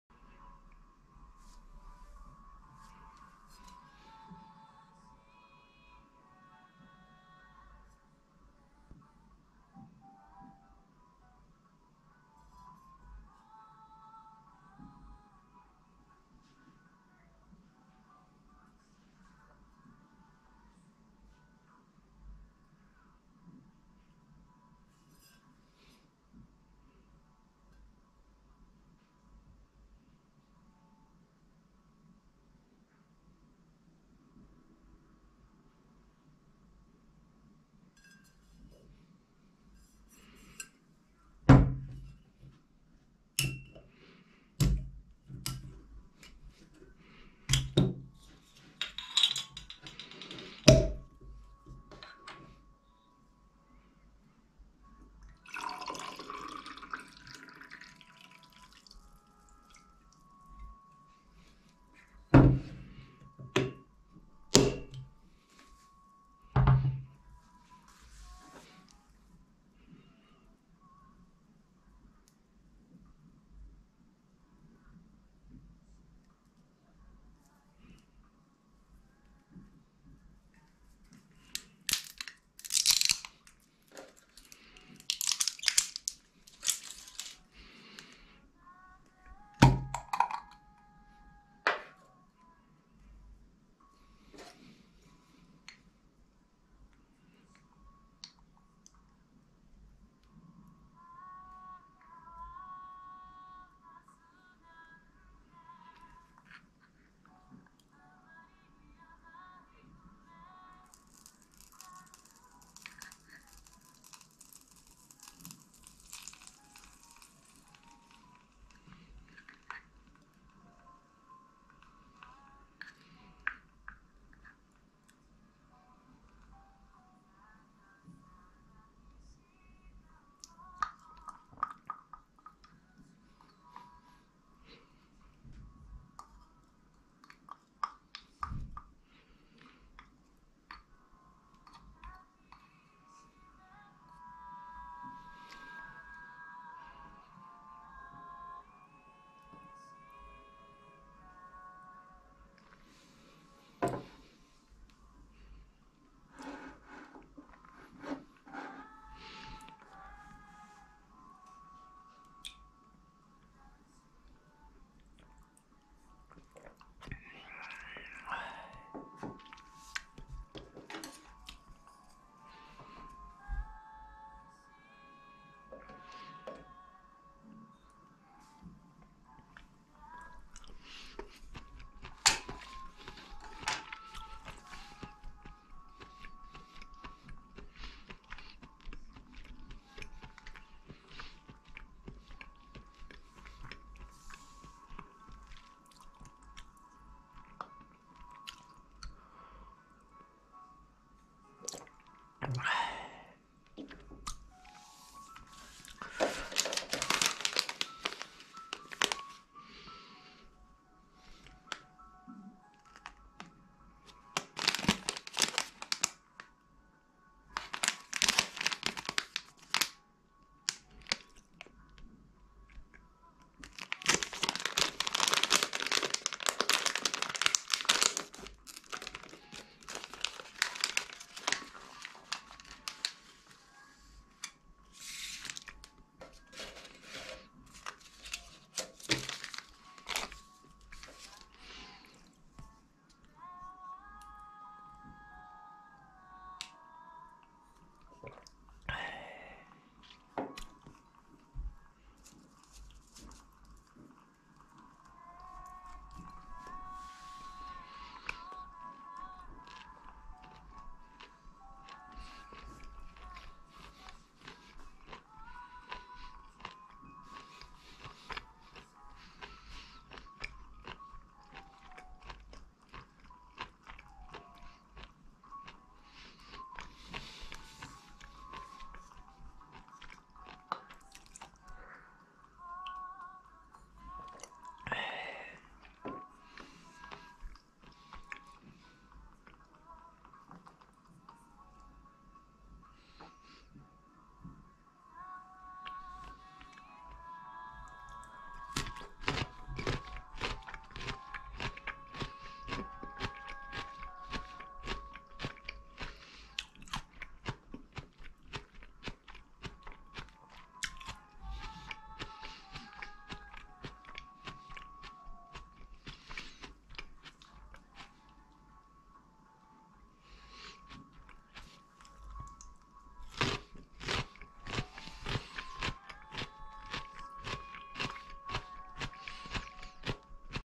VLOG 一人飲み 日本酒 羽根屋 純米吟醸 富山県 SAKE Haneya Junmai Ginjo Toyama 사케 하네야 준마이긴조 토야마 ASMR
一人飲み 日本酒 羽根屋 純米吟醸 富山県 ホタルイカ バーニャカウダ 47都道府県日本酒旅
SAKE Nihonshu Haneya Junmai Ginjo Toyama Baby octopus Bagna cauda
사케 니혼슈 하네야 준마이긴조 토야마 꼴뚜기 바냐카우다
ASMR